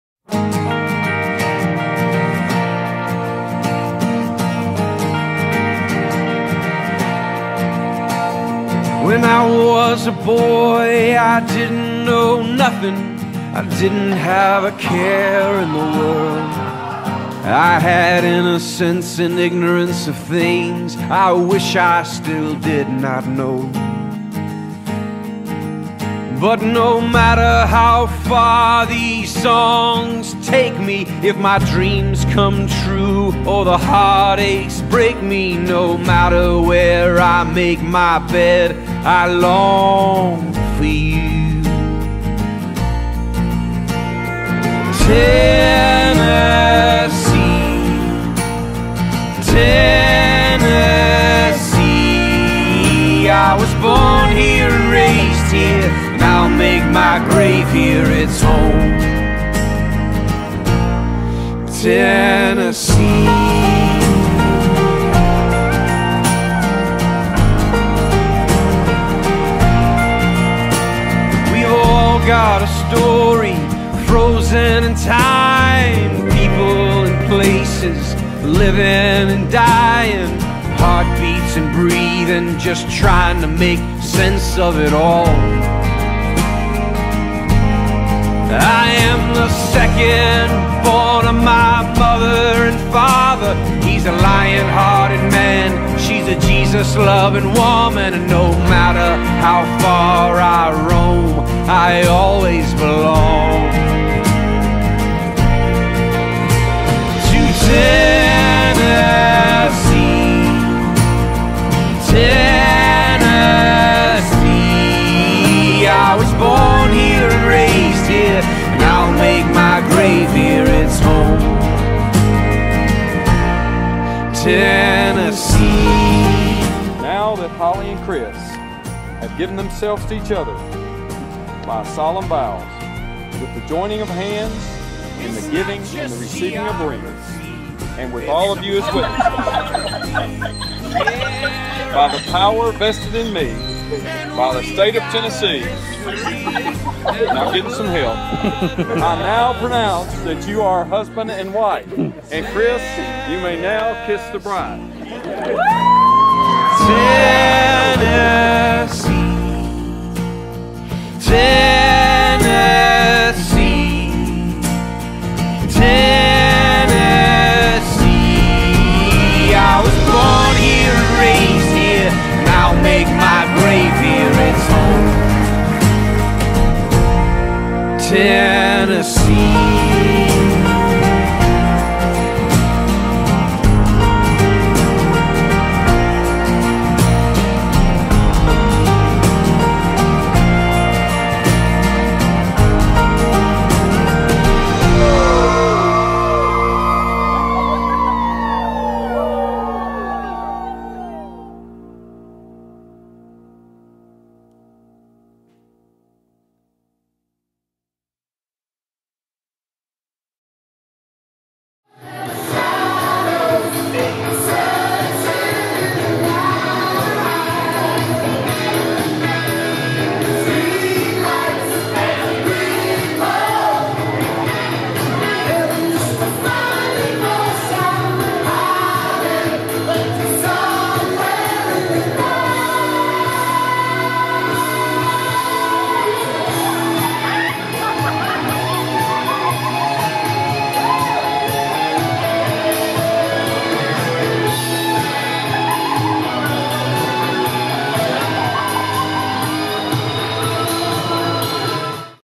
A donkey interrupted this ceremony!
Video 117 of 365:

I have known Hollie for years and was excited to film her and Chris' Big Day! This was also the first time someone had a UT helmet and wore it! These people were obviously not UT fans. Hehe. I think the donkey during the ceremony was a fan too!